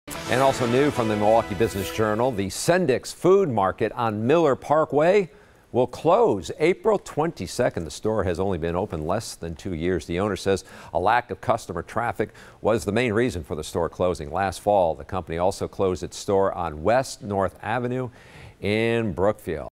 Report: Sendik's to close grocery store in West Milwaukee
A Milwaukee-based grocery chain is closing a location that opened back in 2016.